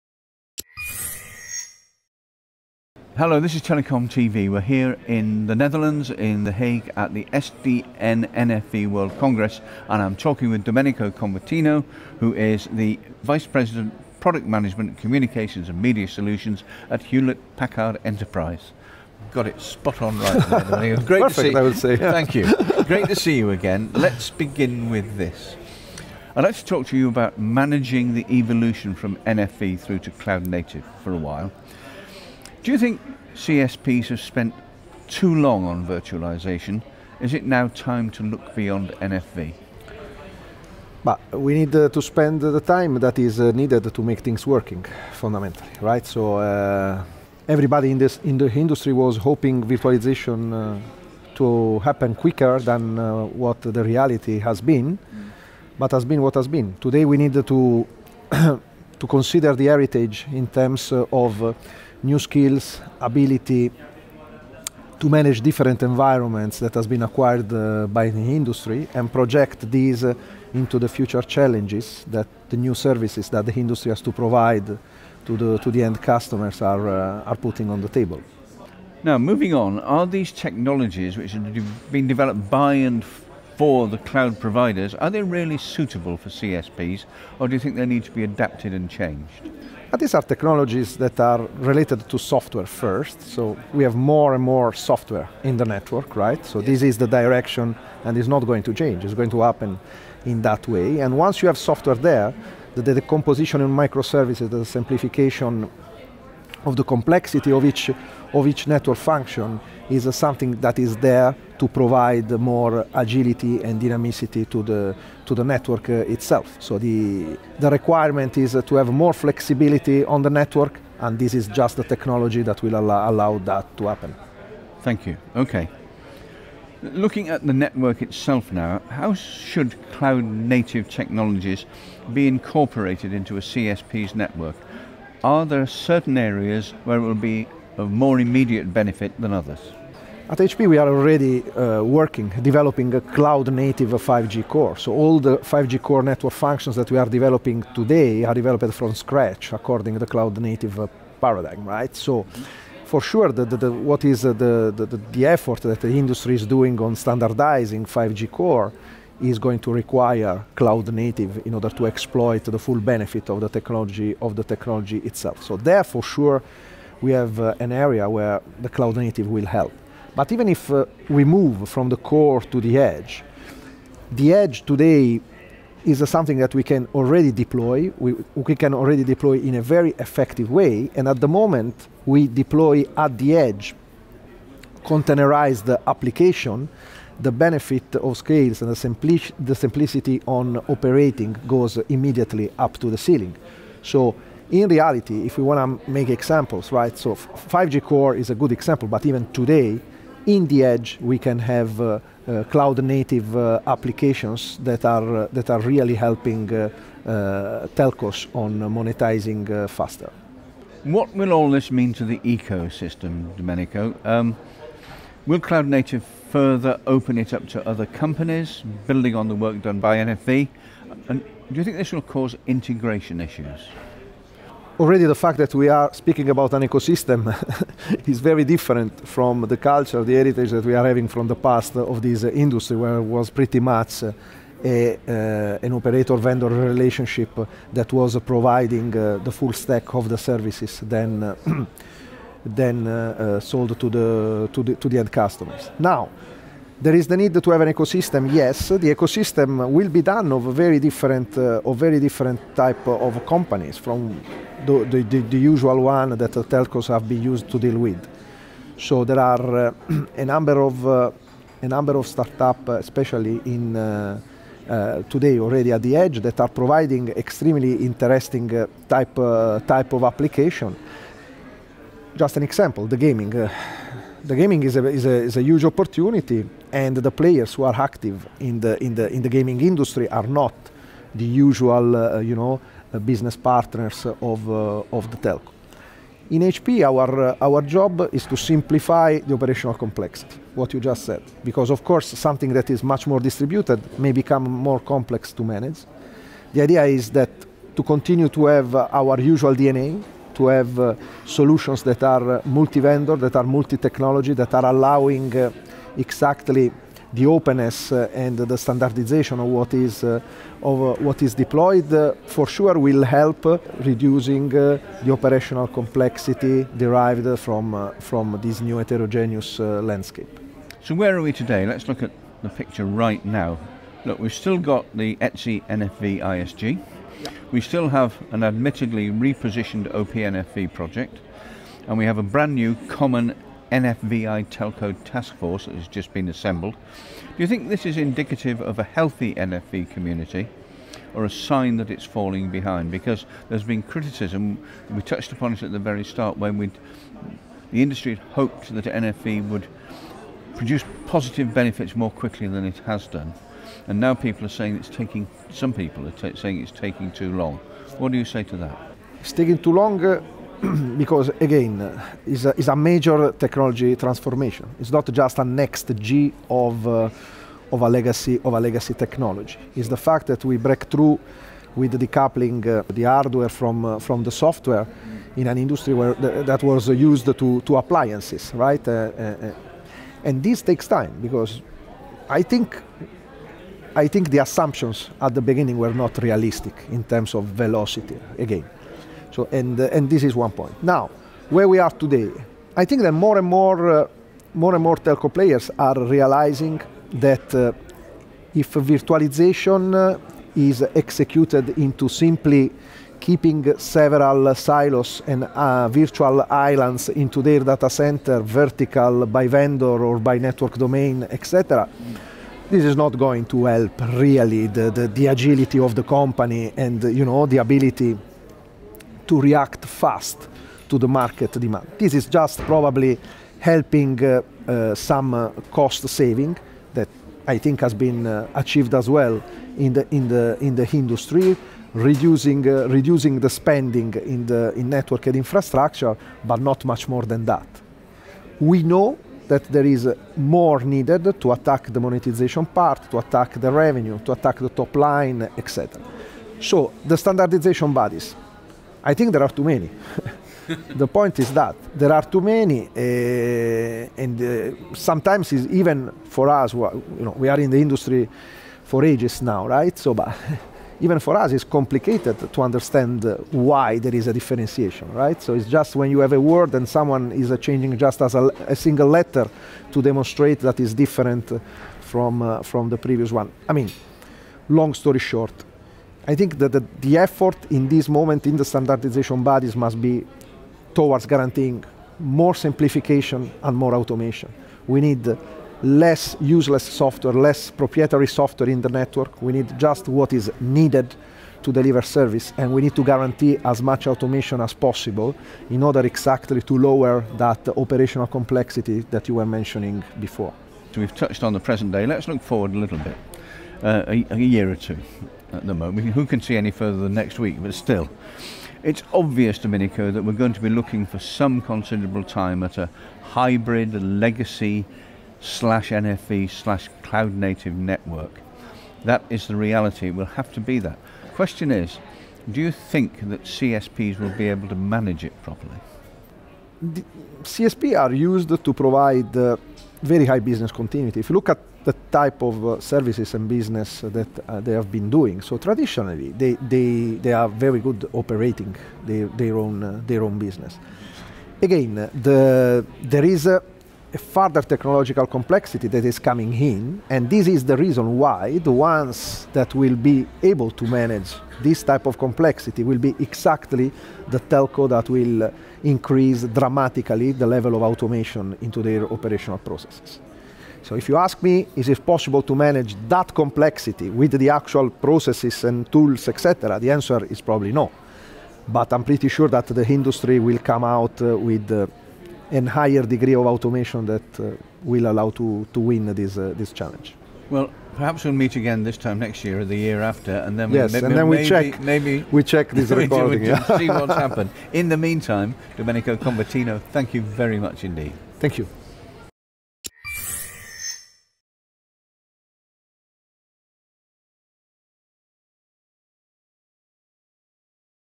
Too many standards cooks spoiling the virtual broth?
Rumbles and grumbles are now echoing around the market to the effect that telcos, operators and CSPs have spent too long on virtualisation and must now accept that it is time to look beyond NFV. Everyone in the industry was hoping and expecting that transformation would happen quickly, but this has not been the reality.

Domenico Convertino accepts that the widespread adoption of NFV has taken longer than initially envisaged but adds that the ongoing network transformation project has been so important, so massive and so globally all-encompassing that the original assumptions about the ease and speed of its deployment were simply not realistic. But then hindsight is always in ‪20:20‬ vision.

It has taken a while, and some say it has taken far too long, for all telcos and CSPs to come to the realisation and acceptance that to introduce virtualisation whilst continuing to retain data, services and applications in separate silos or virtual islands in data centres because of an attachment to or reliance on a particular vendor's equipment, or by keeping them in particular network domains, negates the point of introducing transformation in the first place.

Certainly cost savings can be achieved under such a scenario but it militates against telcos in terms of agility and the in-built ability to react quickly to changing market demands that full virtualisation and a proper cloud native environment provides.

Domenico Convertino points out that telcos, operators and CSPs have learned a great deal and acquired important new skills as a direct result of solving the problems they have faced whilst grappling with transformation SDN, NFV and the cloud. Furthermore, the amount of software in networks is steadily increasing and that is already permitting the simplification of  some of the complexities of each network function.

Mr. Convertino adds that the pace and breadth of transformation has been impeded rather that hastened by the existence of too many standards bodies. He emphasises that the key tenets of virtualisation have always been to increase simplicity, enhance flexibility and introduce automation and that standards bodies really must keep those objectives in mind at all times in their deliberations.

Basically, the less proprietary software there is in a transformed network the better. What is required is no more than necessary to deploy services and to ensure as much automation as possible to reduce operational complexity. However, reaching such a desirable stats is still some way off.